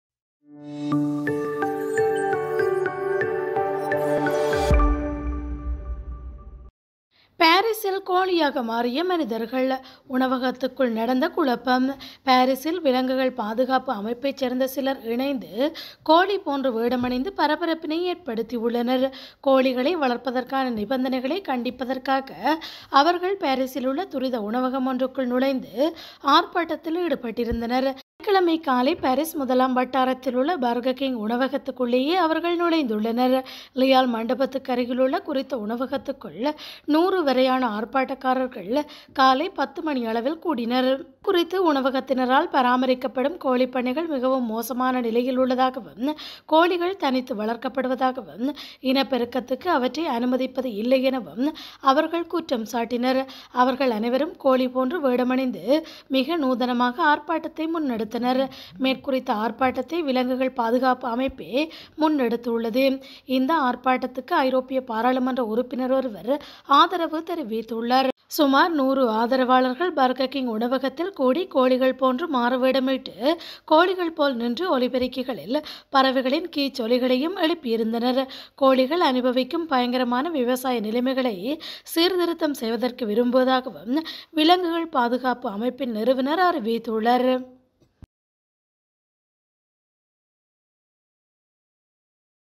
பாரிஸில் கோழியாக மாறிய மனிதர்கள் உணவகத்திற்குள் நடந்த குழப்பம்
பாரிஸில் விலங்குகள் பாதுகாப்பு அமைப்பைச் சேர்ந்த சிலர் இணைந்து கோழி போன்று வேடமணிந்து பரபரப்பை ஏற்படுத்தியுள்ளனர்.